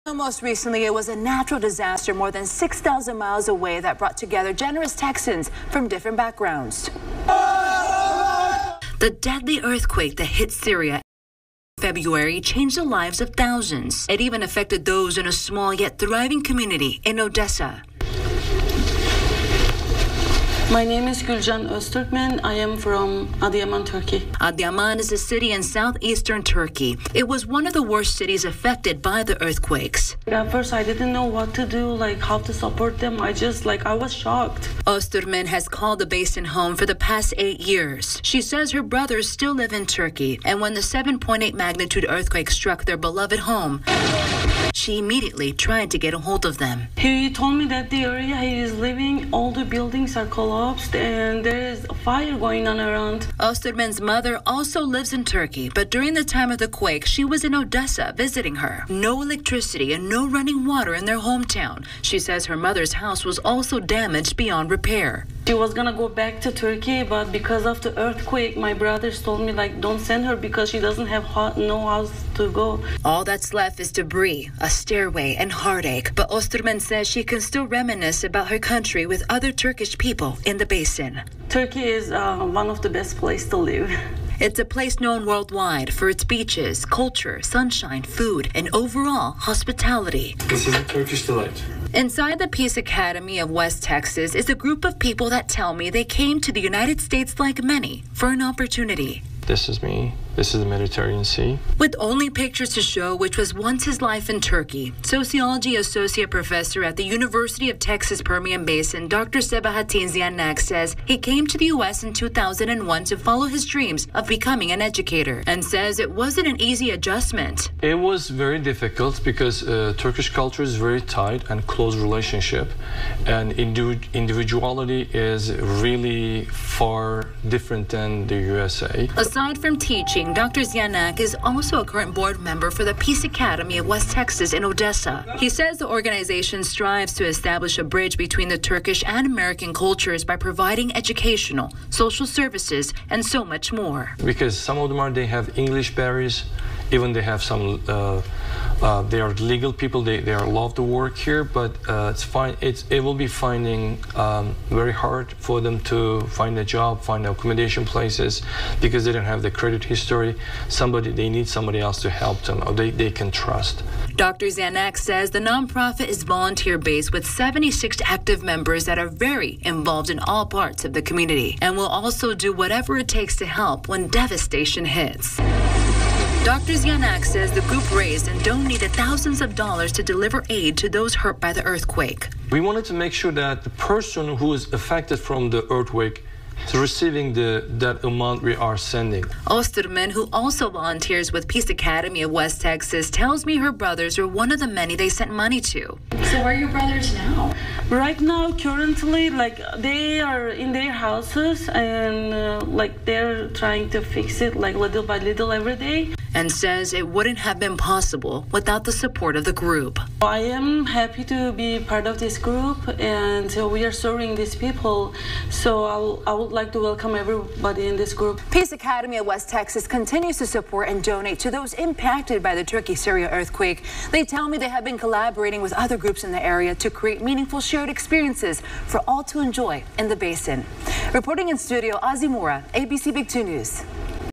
Local Turkey aid efforts
Peace Academy of West Texas goes above and beyond.